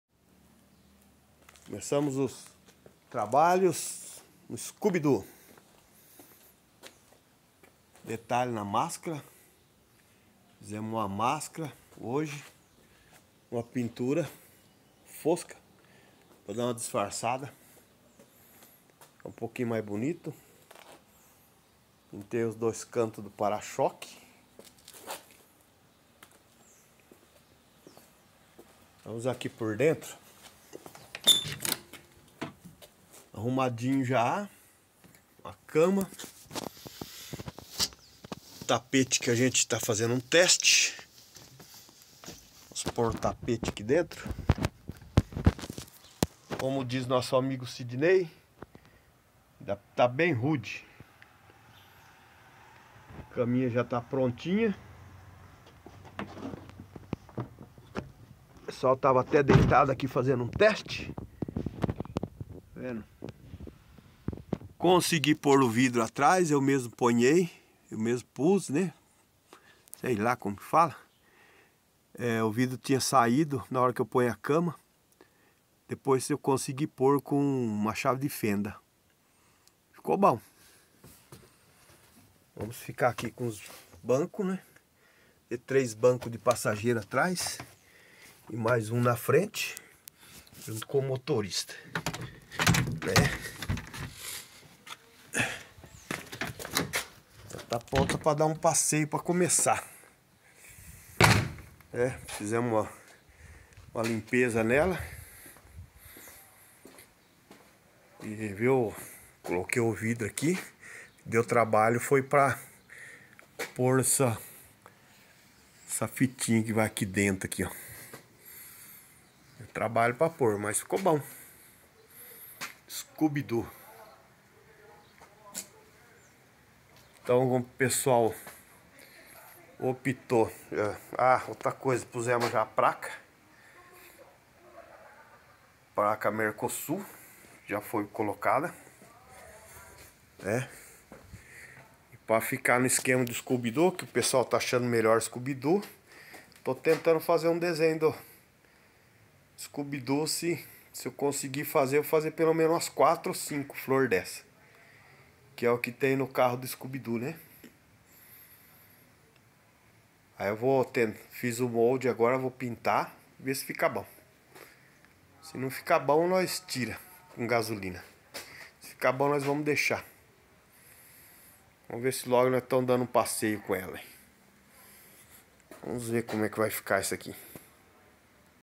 dando um trato em nosso motor home scubi em um furglain 1983
estamos  montando nosso motor home em um furglain 1983 feito todo de fibra em uma carroceria de f 1000